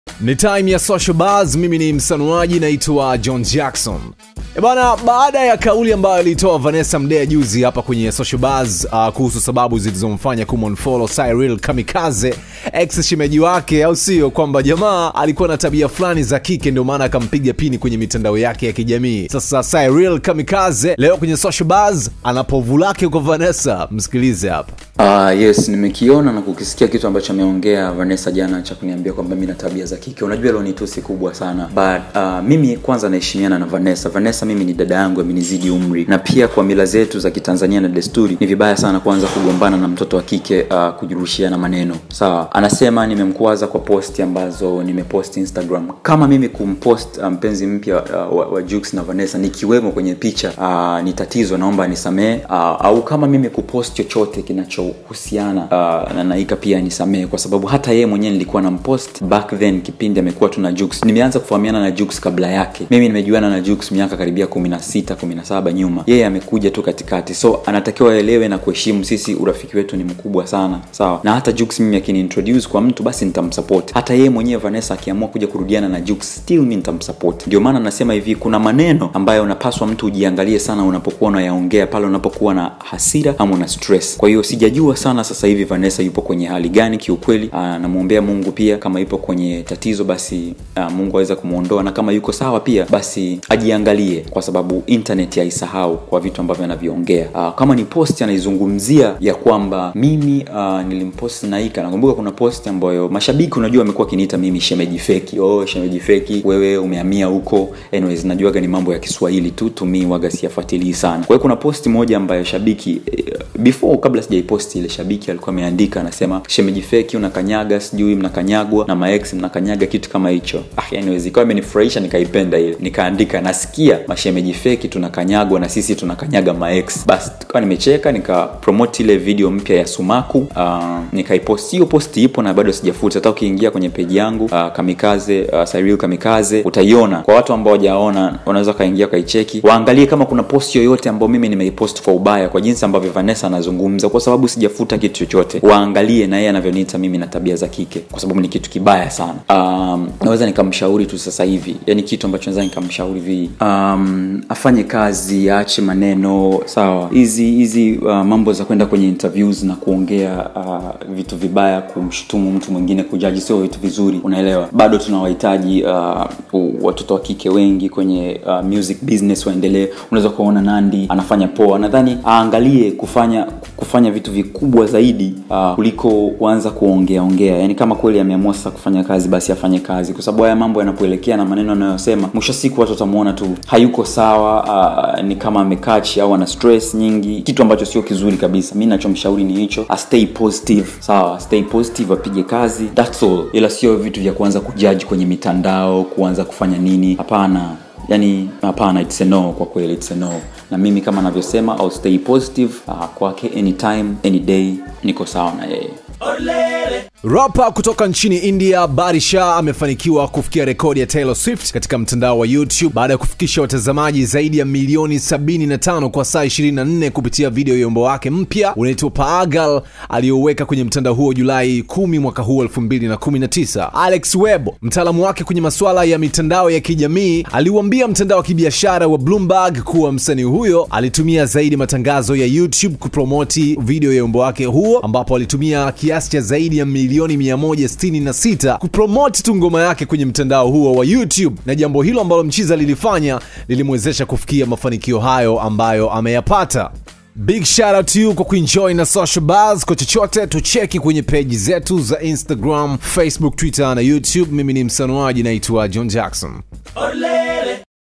VANESSA AMENIKOSEA HESHIMA/AFANYE MAISHA YAKE/DEMU WA JUX NI MKALI:CYRILL KAMIKAZE
Baada ya Vanessa Mdee kumtaja Cyrill kuwa anatabia za kike ndio maana akamu-unfolollow kwenye instagram,msanii huyu ameibuka na kumtolea povu zito Vanessa.Sikiliza hapa.